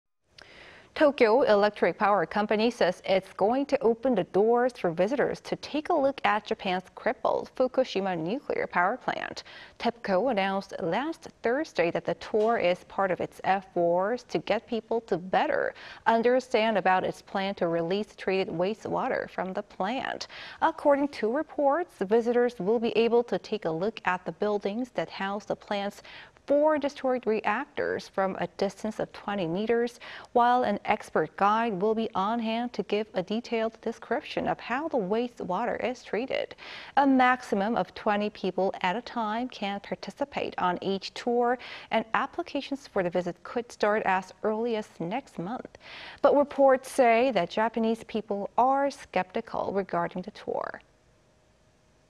TEPCO to allow visitors to tour crippled Fukushima nuclear power plant site
"후쿠시마 원전 투어" 도쿄전력, 일반인 대상 투어 추진

Tokyo Electric Power Company says it's going to open the doors for visitors to take a look at Japan's crippled Fukushima nuclear power plant.
TEPCO announced last Thursday that the tour is part of its efforts to get people to better understand about its plan to release treated wastewater from the plant.
According to reports, visitors will be able to take a look at the buildings that house the plant's four destroyed reactors. from a distance of 20 meters. while an expert guide will be on hand to give a detailed description of how the wastewater is treated.
A maximum of 20 people at a time can participate on each tour, and applications for the visit could start as early as next month.
But reports say that Japanese people are skeptical regarding the tour.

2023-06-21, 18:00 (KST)